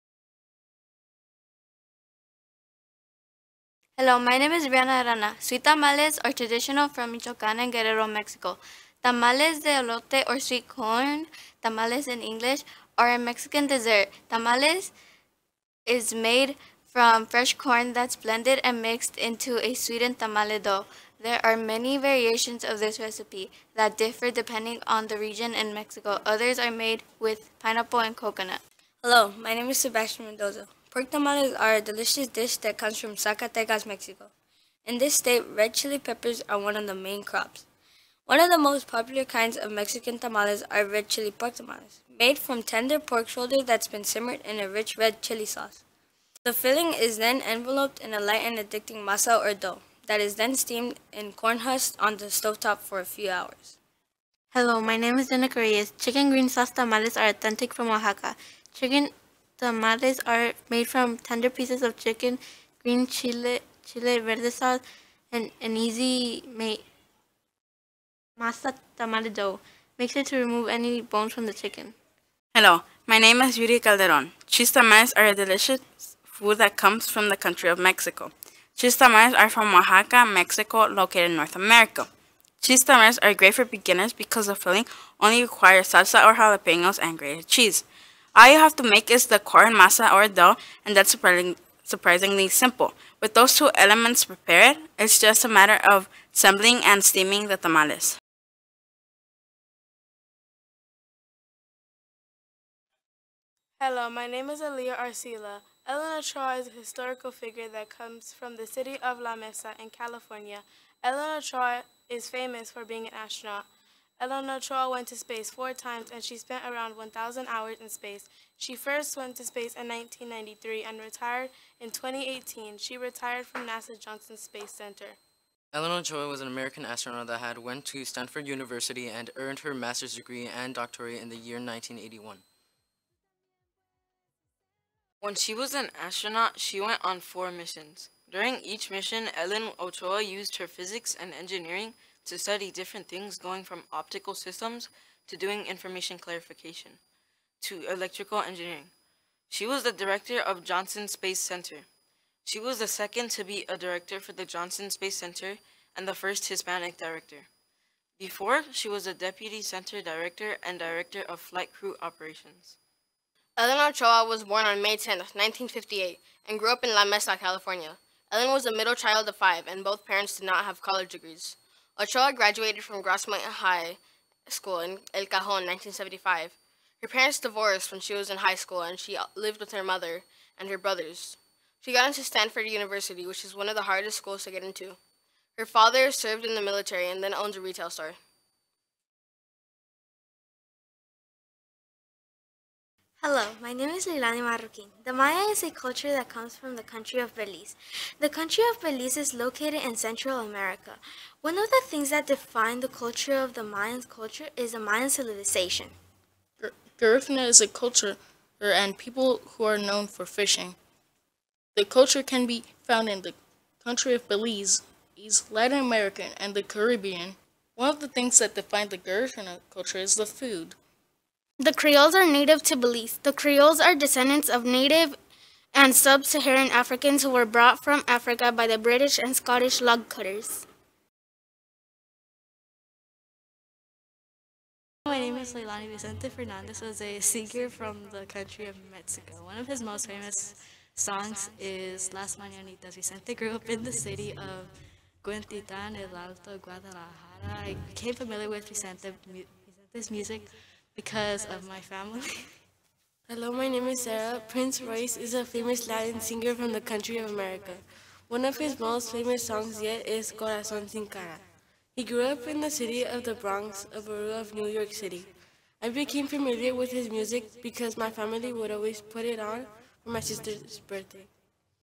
Room 59 final
Russell Elementary Room 59 covers Latin American History Month